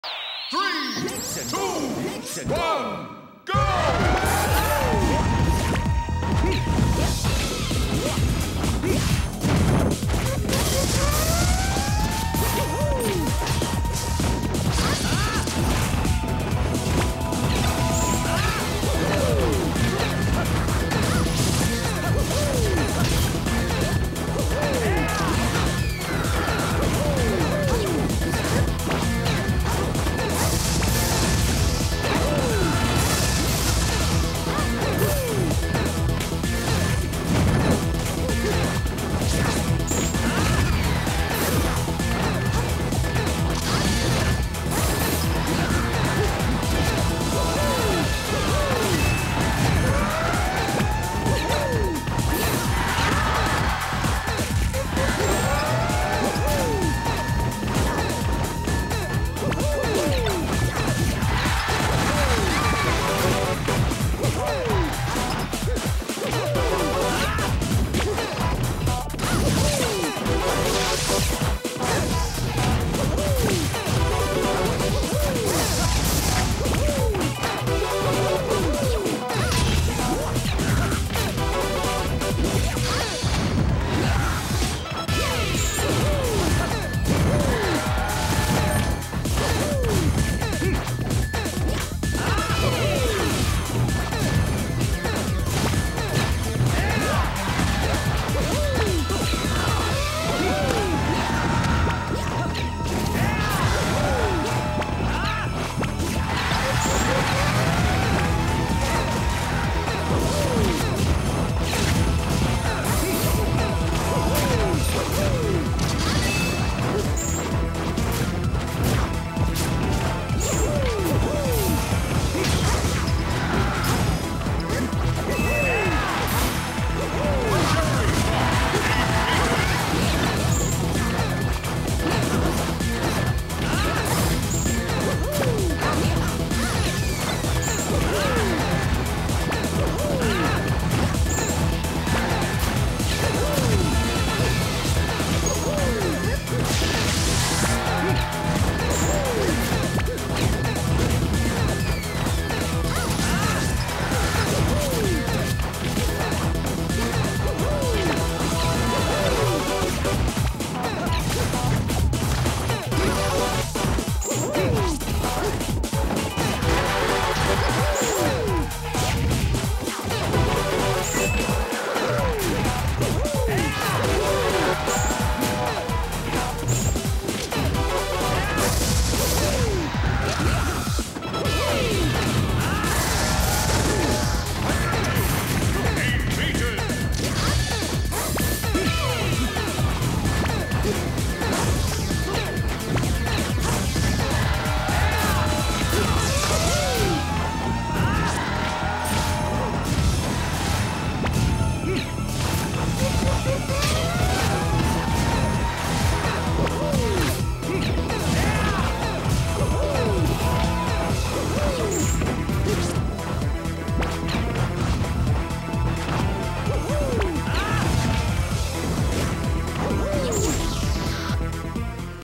LT: ALSM(Link) & Dakpo(Mario) vs Infinity(Mario) & Ultimate Razer(Zelda) 1
Low Tier Side event at
ALSM's Texas Crusade Series
6/9/12